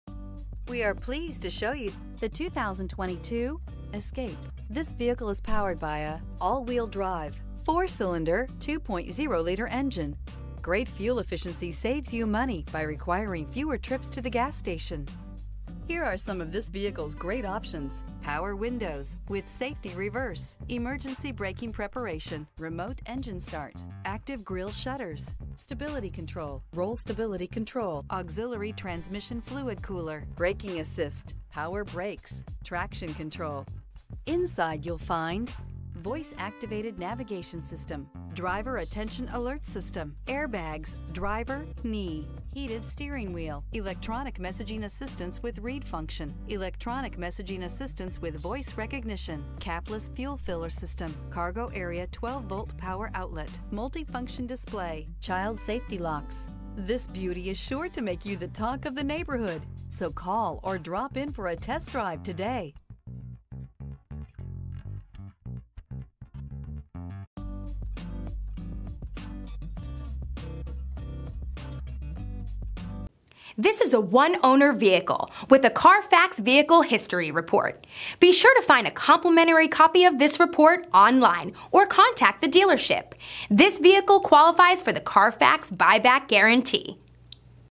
2022 Ford Escape Kingsport TN 7702P
2022 Ford Escape Titanium

2733 East Stone Dr.

CARFAX One-Owner. Clean CARFAX.brbrAWD. White Metallic 2022 Ford Escape Titanium AWD 8-Speed Automatic EcoBoost 2.0L I4 GTDi DOHC Turbocharged VCTbrbrRecent Arrival! Odometer is 7450 miles below market average! 22/31 City/Highway MPGbrbrbrPlease contact our internet department with any questions @ (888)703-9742.